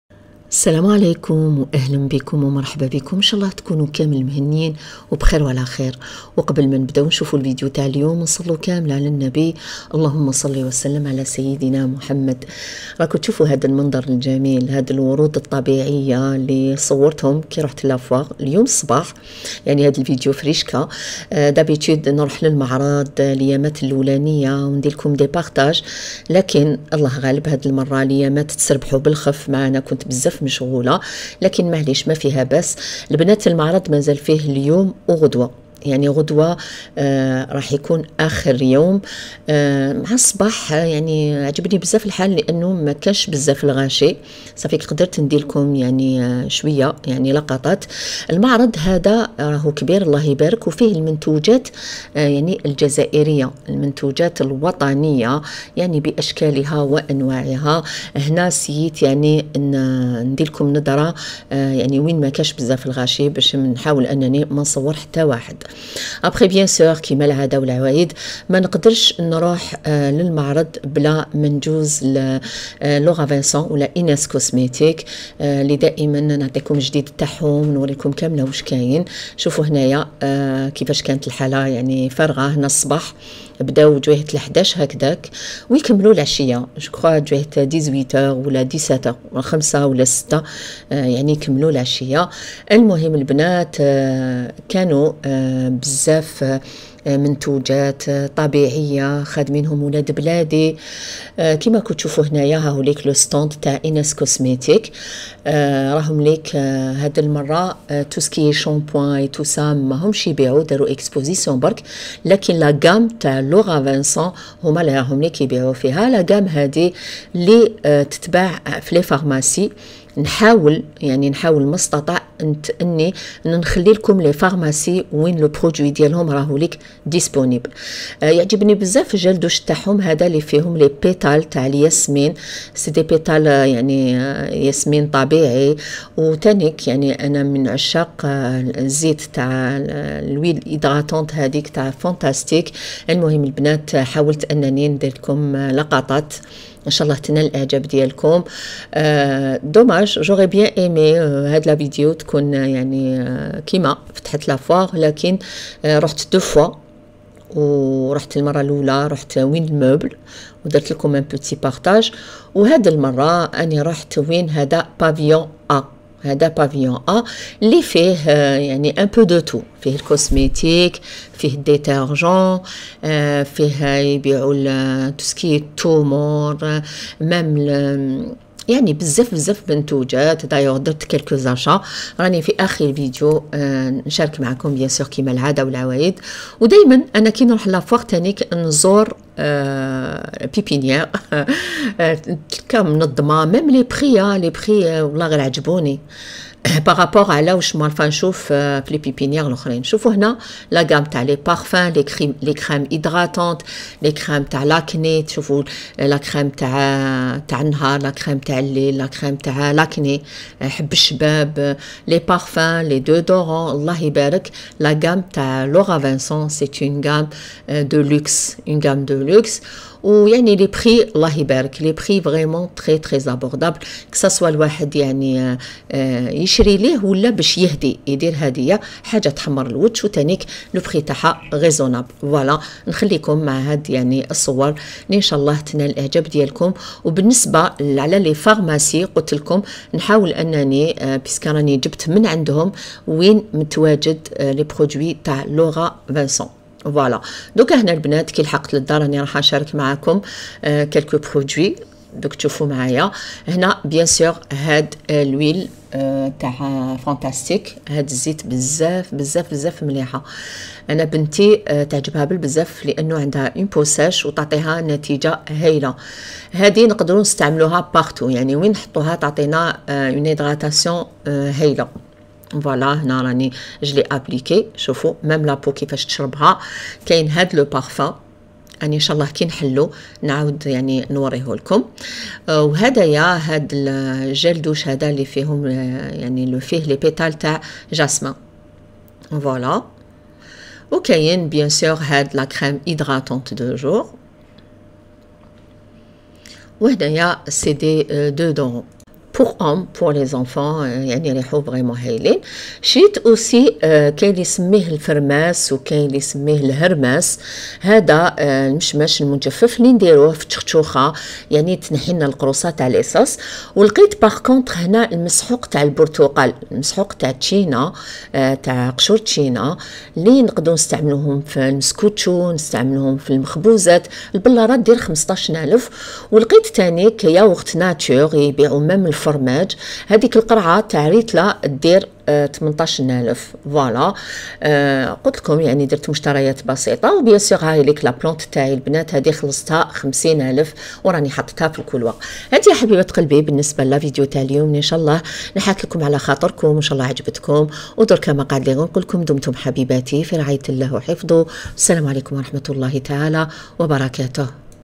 الجمعة و الحال شاب ،تحوسة في معرض الوطني ،منتجات جزائرية 🇩🇿،مشتريات بسيطة،يا الله ارواحو معايا🤗
هاذو هوما الروابط تاع مواقع التواصل تاعي اما الصفحات الاخرى كلها ماشي تاعي